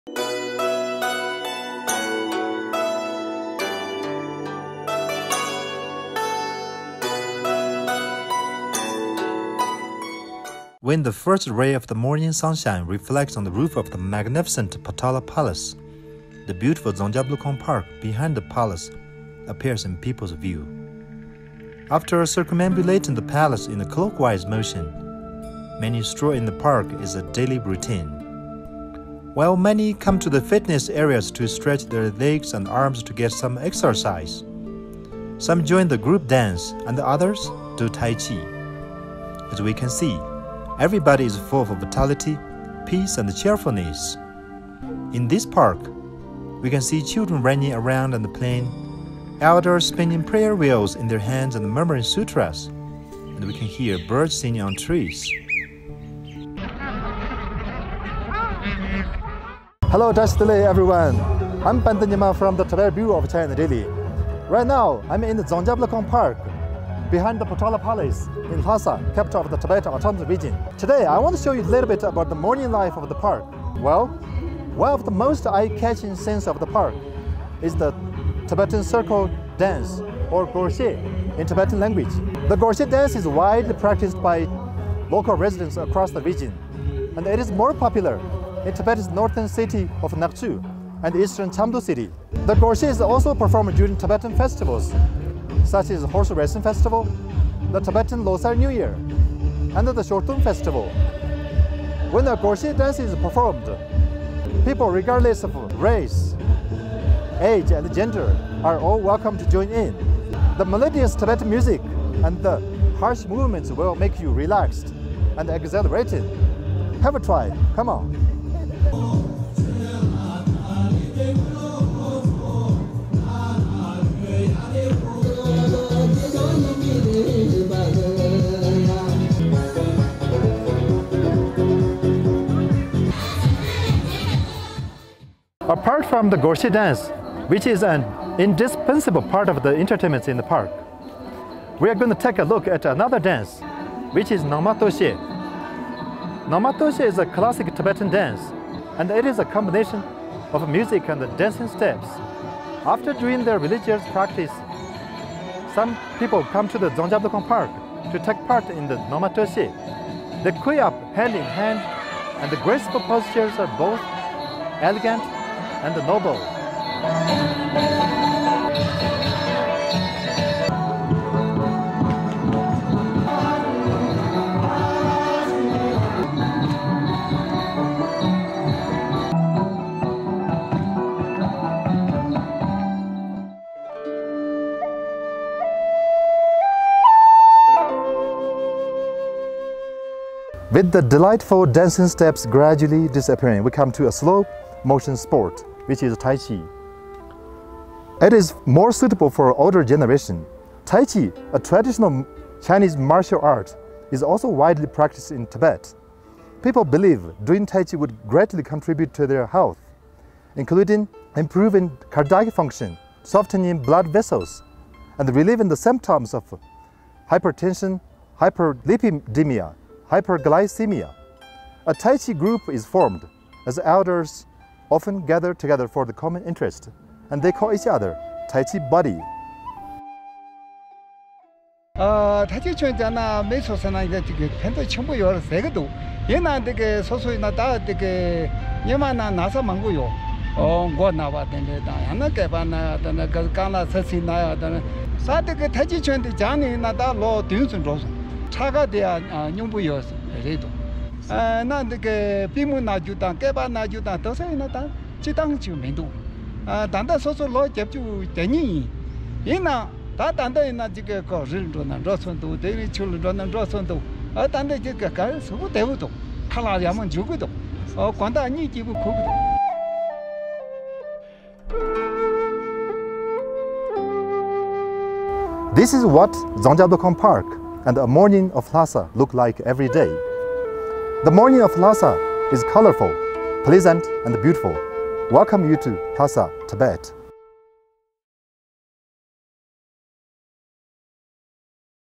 Lhasa's park bustles with activities
The Dzongyab Lukhang Park behind the PotalaPalace in Lhasa, capital of the Tibet autonomous region, is one of the most eye-catching places in the city. The morning scene of the park is bustling, people come to the park for different purposes, including doing religious practices, exercises, dancing, singing, gatherings, and feeding the birds.

The Tibetan traditional circle dance, or Gorshai in Tibetan language, is widely practiced by local residents across the region, and it is more popular in Tibet's northern city of Nagchu and eastern Chamdo city. Gorshai is performed during Tibetan traditional festivals, such as horse racing festivals, the Tibetan Losar New Year, and the Shoton Festival.

Apart from the Gorshai dance, Nangma Doishai is also a hot spot in the park.
As a classic Tibetan dance, Nangma Doishai is a combination of music and dancing steps. People queue up hand in hand to take part in the Nangma Doishai, and the graceful postures are both elegant and noble.

In addition to the Tibetan Gorshai and Nangma Doishai dances, taichi, a traditional Chinese martial art, is also widely practiced in Tibet. People believe doing tai chi would greatly contribute to their health, including improving cardiac function, softening blood vessels, and relieving the symptoms of hypertension, hyperlipidemia and hyperglycemia.